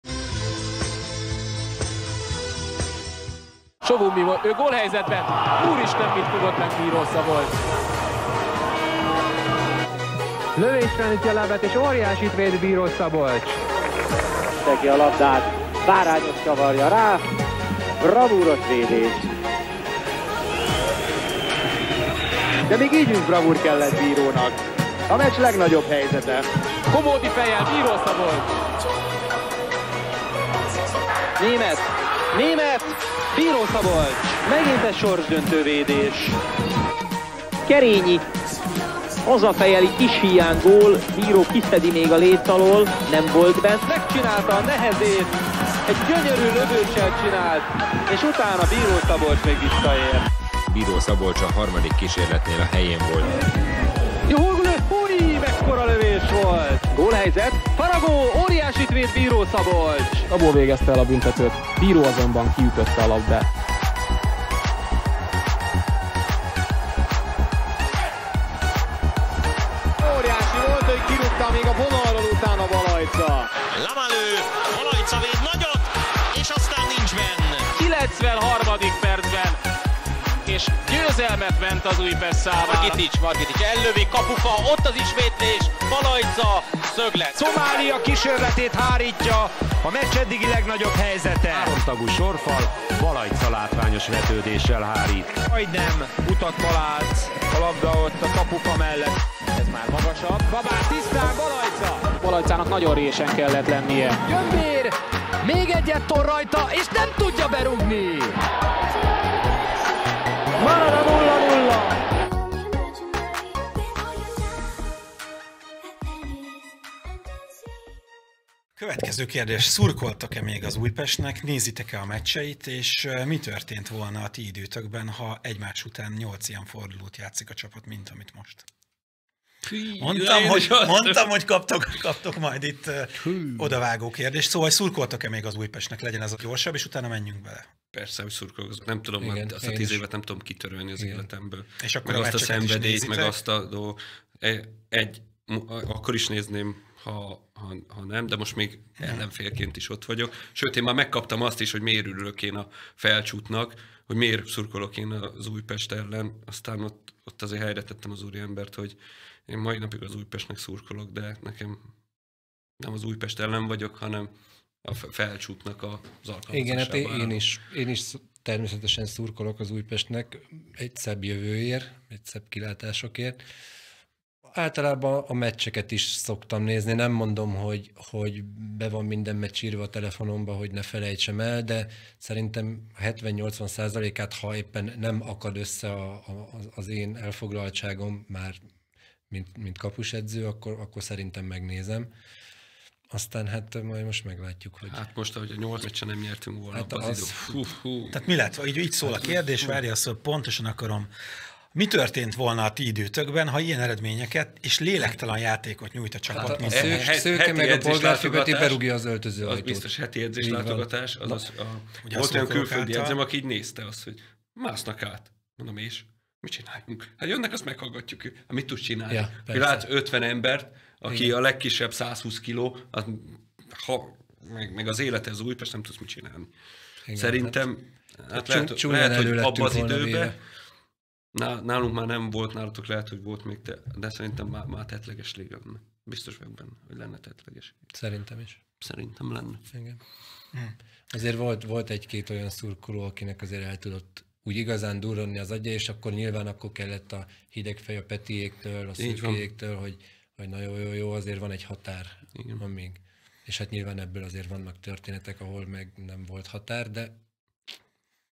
Balajcza, Bíró: Az Újpest kapusa voltam - előzetes
A 2 kapus legnagyobb védései, és egy rövid előzetes a hamarosan érkező interjú videóból.